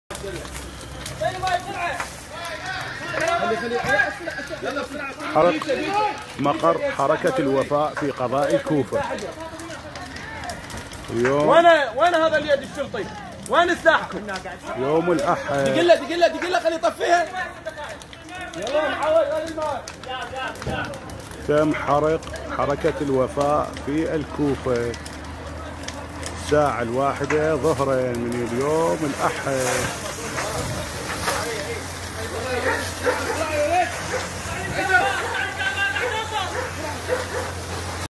احتراق مقر حركة الوفاء التابعة لرئيس كتلة النصر النيابية عدنان الزرفي في مدينة
احتراق مقر حركة الوفاء التابعة لرئيس كتلة النصر النيابية عدنان الزرفي في مدينة الكوفة بمحافظة
ـــــــــــــــــــــــــــــــــ